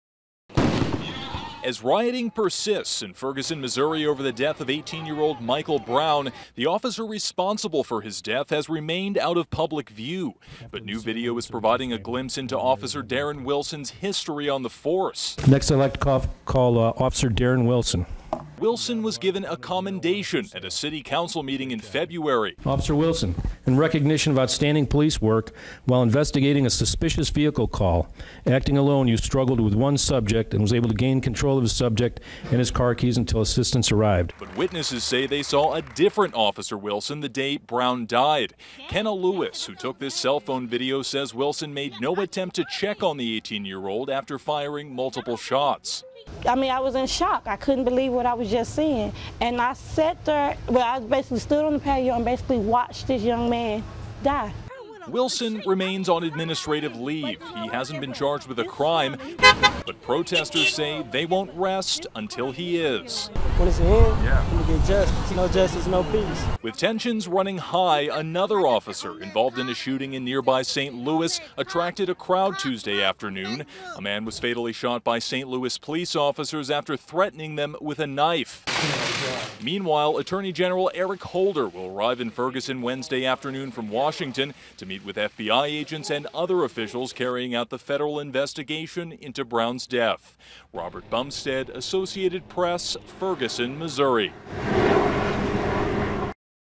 Video Shows Ferguson Cop Months Before Shooting
Video obtained by the Associated Press shows officer Darren Wilson receiving a commendation months before the shooting of Michael Brown. Residents who took cellphone video right after the shooting say they got a different image of the officer. (Aug. 19)